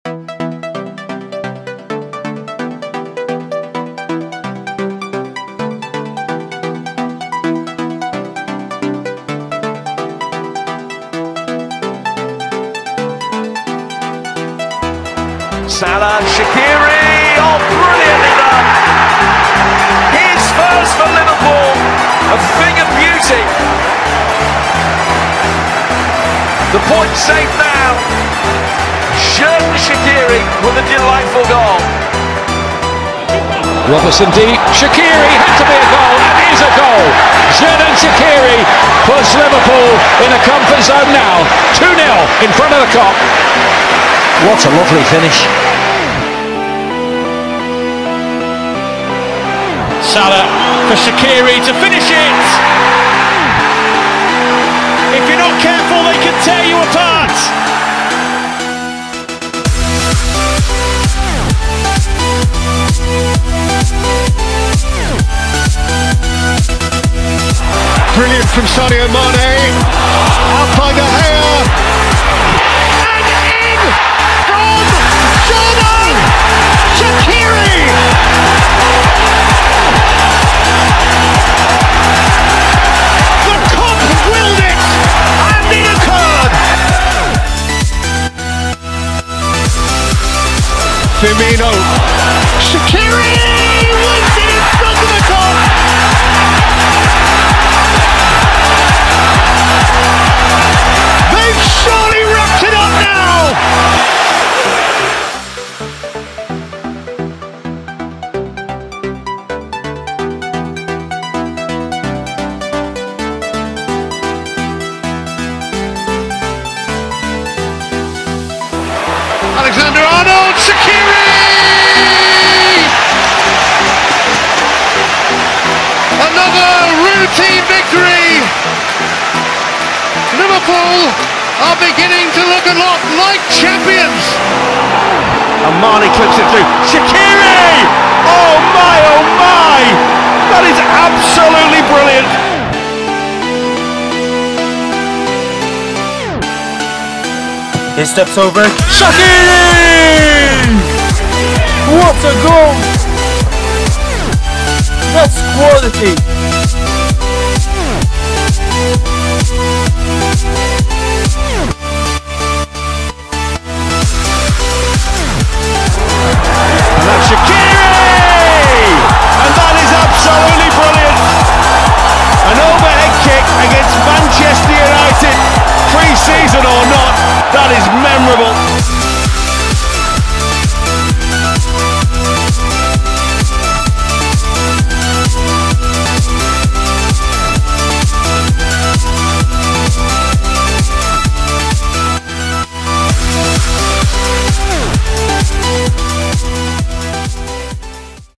Xherdan Shaqiri ALL GOALS for Liverpool - Anfield's Swiss HERO 🚀
Big Shaq was signed by Jurgen Klopp in 2018, and he had a very important role in their Champions league triumph that season! Short but explosively quick, he will always be remembered for his rocket long shots, huge calves and of course the double against Man Utd!!

The Magic Dwarf is a Liverpool Cult hero, cementing his status by also scoring against Everton in the Merseyside Derby.
He also scored an unforgettable overhead kick against United in a preseason friendly in front of a crowd of 100,000 people!

He also scored a thunder freekick against lincoln in the league cup!

The powercube will never be forgotten at Anfield!
SHAQ ATTAQQ!

Thanks for Everything Xherdan Shaqiri! Good Luck at Olympique Lyon! Welcome to Chicago Fire FC!